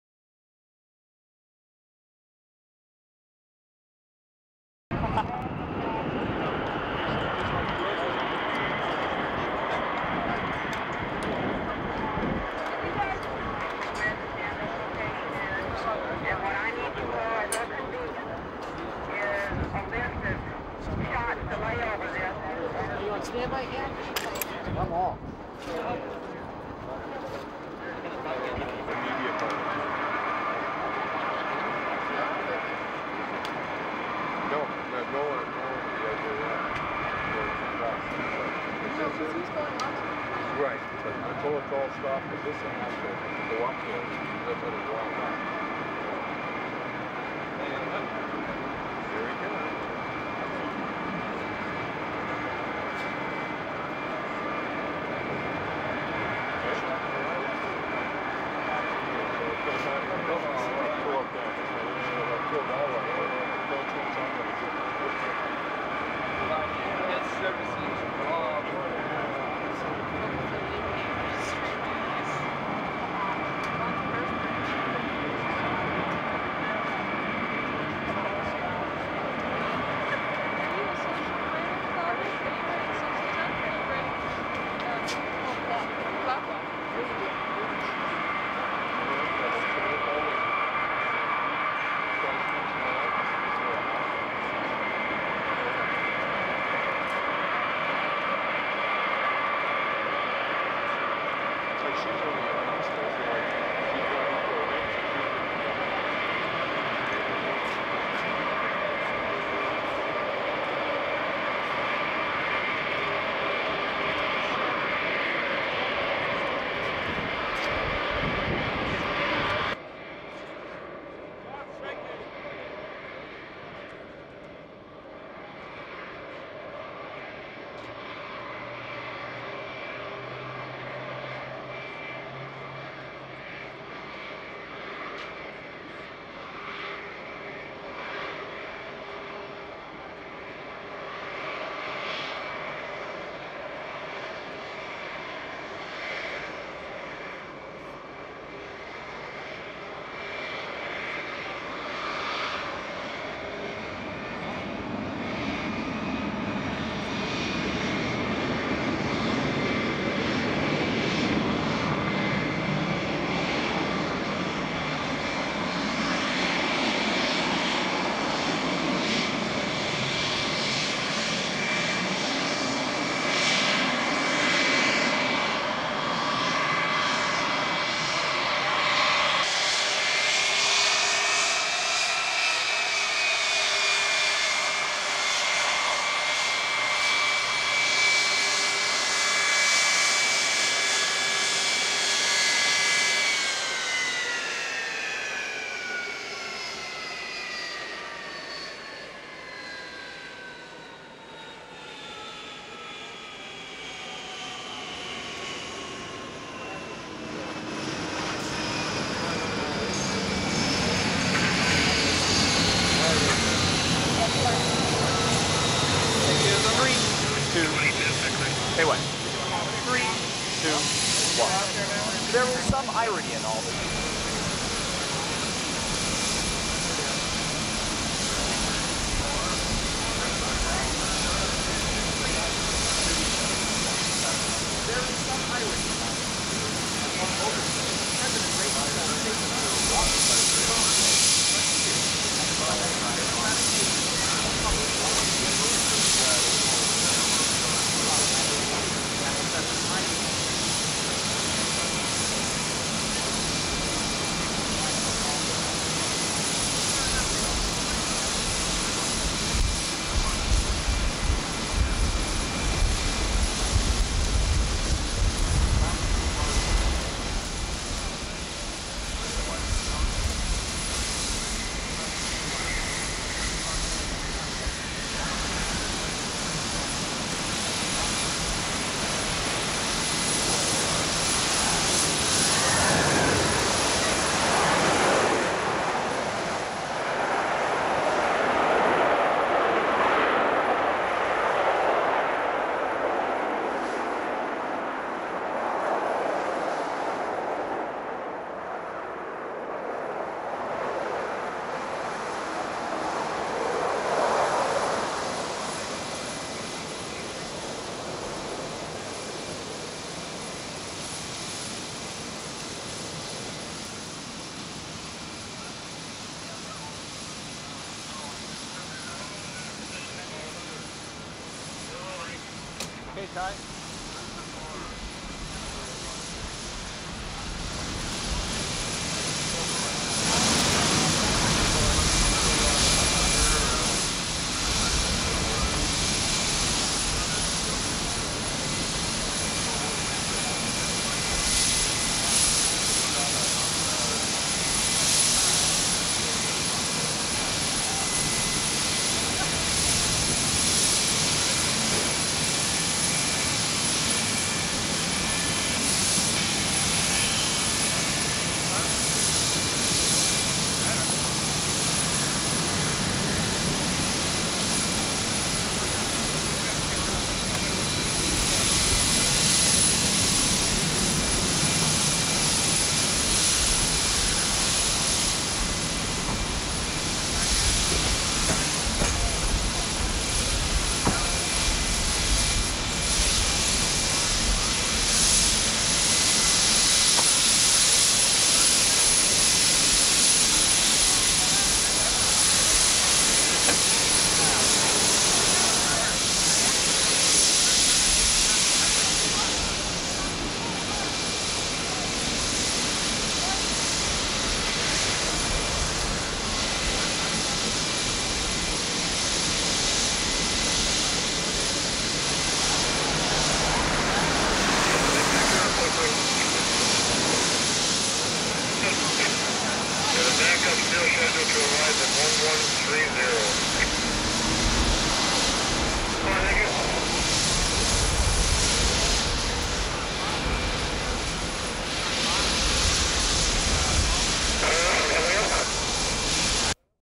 President Reagan Arrives in Toronto, Canada for Economic Summit on June 19, 1988
Full Title: Toronto Summit. President Reagan and Nancy Reagan Arrive on Air Force One at Lester B. Pearson International Airport on June 19, 1988

Transcript: N/A

Production Date: 6/19/1988

Access Restriction(s):Unrestricted
Use Restriction(s):Unrestricted